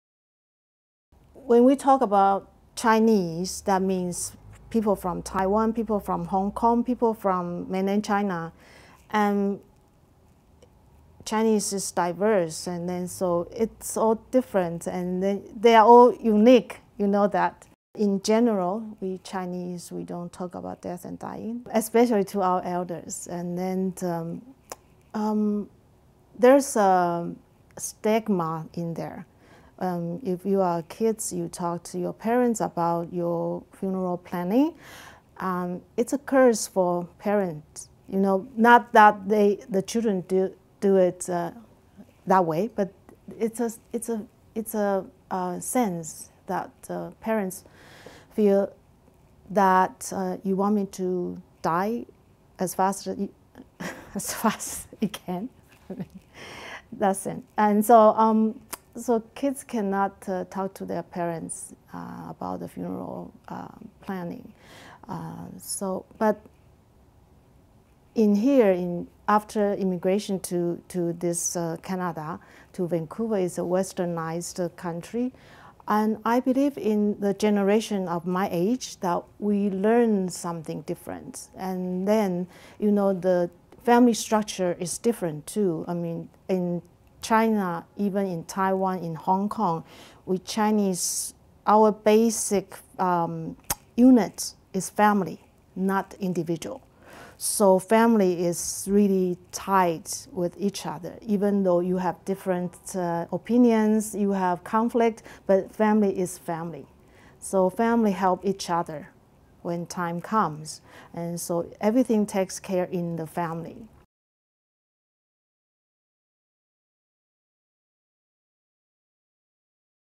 Marisa: We don't talk about death
In this video by Canadian Virtual Hospice, Marisa talks about the challenge of discussing dying.